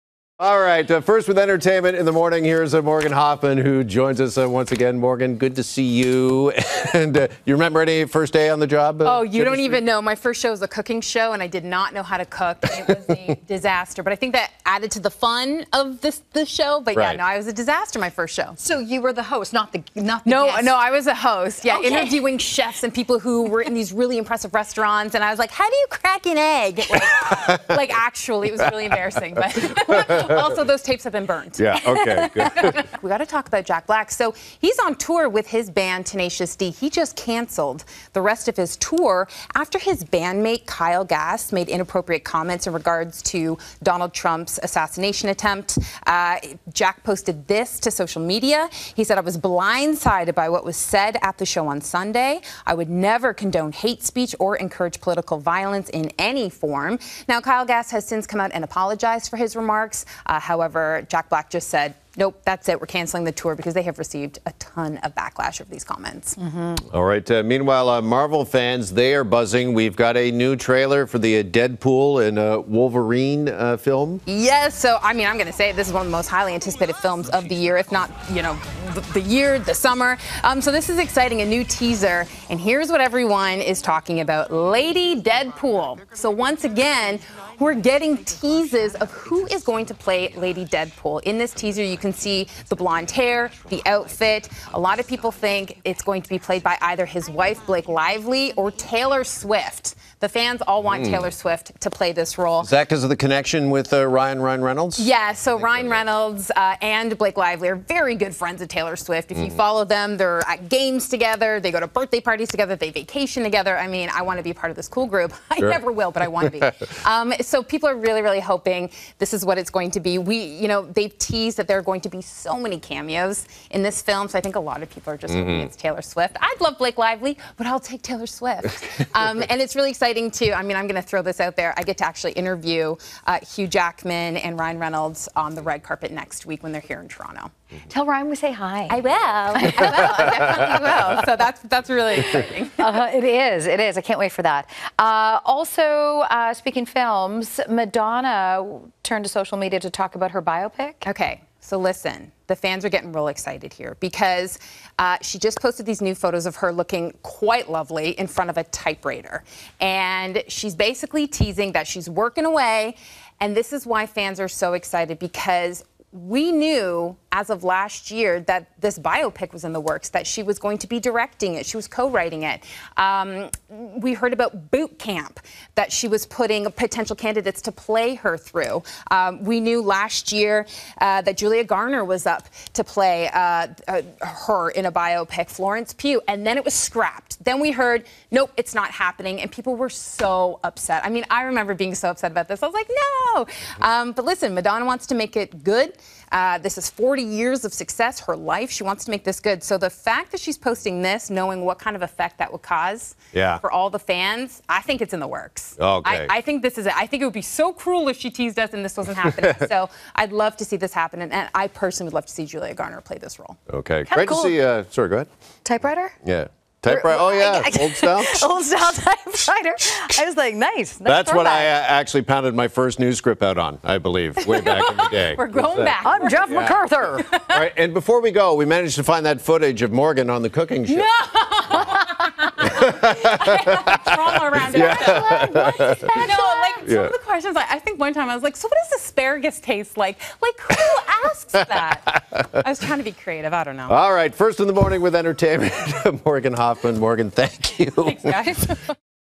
Jack Black cancels Tenacious D tour after bandmate jokes about Trump shooting
We’re First in the Morning with Entertainment with Morgan Hoffman, as she brings us the latest buzz. She talks about Ingrid Andress speaking out after her Home Run Derby performance, Jack Black's tour hits headlines with bandmate Kyle Gass's comment about Trump. She also discusses Marvel fandom as the new 'Deadpool/Wolverine' trailer drops. Plus, Madonna shares updates on her biopic, 'Who's That Girl,' which she directs and stars Julia Garner.